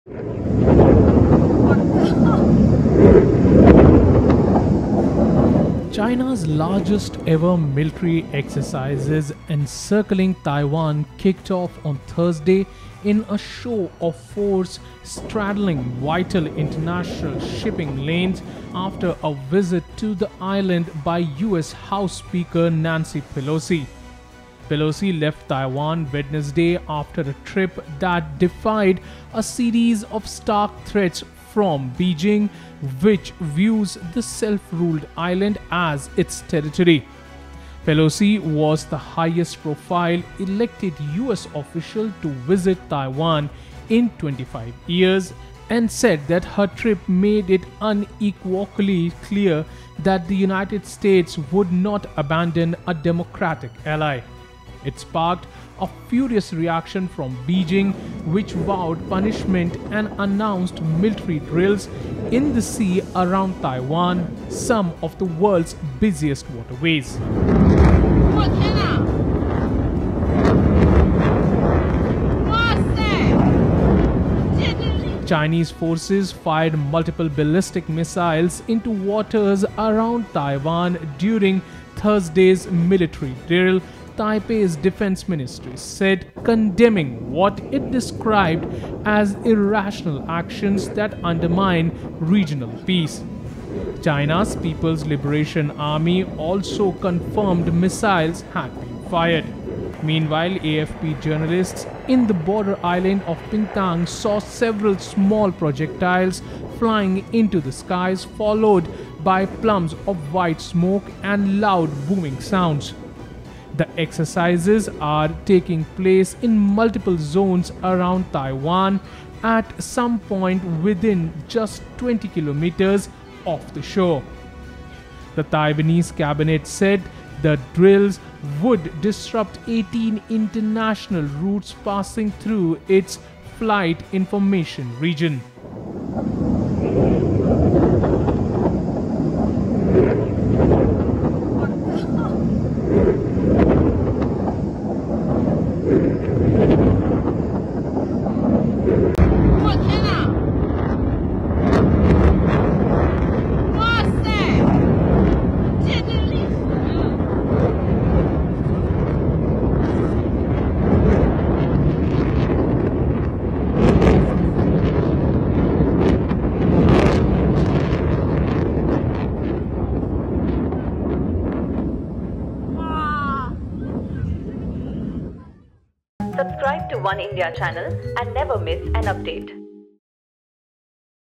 China conducts military drill near Taiwan, fires multiple missiles | Oneindia News *News
China on Thursday conducted military drill around Taiwan, a day after US speaker Nancy Pelosi visited the country. According to reports multiple ballistic missiles were also fired during the drill.